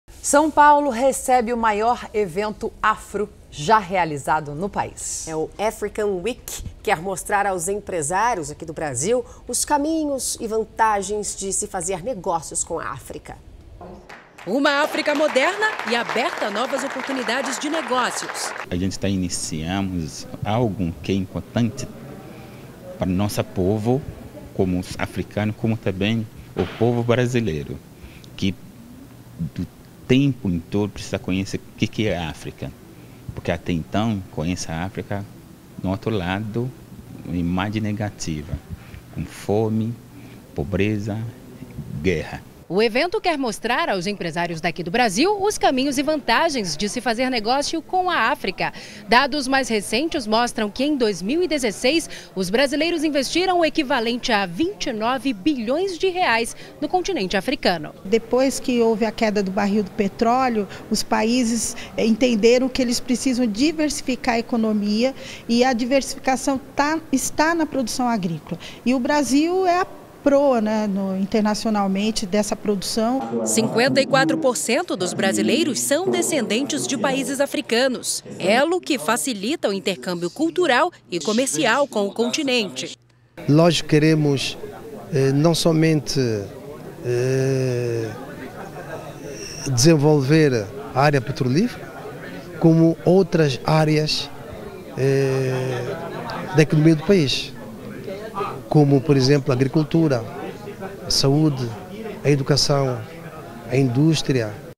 São Paulo recebe maior evento afro já realizado no país
O African Week conta com a colaboração de organizações que auxiliam socialmente e disseminam a cultura africana no Brasil. A iniciativa visa diminuir a desigualdade social e a discriminação, além de abrir um novo horizonte de negócios entre o Brasil e os paises do continente africano.